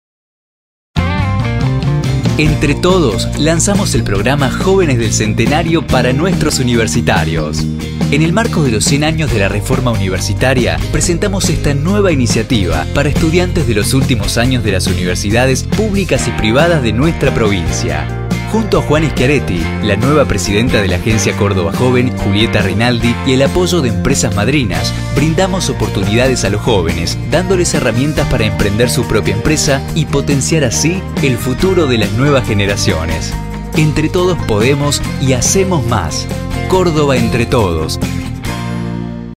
Programa Provincial Jóvenes del Centenario
En el marco del centenario de la Reforma Universitaria, con la presencia del Gobernador Juan Schiaretti presentamos la iniciativa que tiene como propósito profundizar el vínculo entre el Gobierno de Córdoba –por medio de la Agencia Córdoba Joven- con las Universidades emplazadas en nuestra Provincia, sus estudiantes, el
universo productivo y económico, y el mundo del emprendedorismo.

El objetivo fundamental es favorecer la inserción laboral de los jóvenes cordobeses a través del fomento de una política pública que favorezca su desarrollo personal y académico,
permitiéndoles contar con las herramientas, habilidades y capacidades necesarias para generar autoempleo y emprender la propia empresa.

Se trata del desarrollo de proyectos socio-comunitarios a través del las áreas de Extensión de las universidades; una gran variedad de capacitaciones y talleres; y el acceso a prestamos de hasta 100 mil pesos con 0% de interés; como también la entrega de 50 subsidios no reembolsablesde de hasta $200 mil, para proyectos innovadores y de base tecnológica.

El programa estará disponible a partir de la apertura del ciclo lectivo del 2018.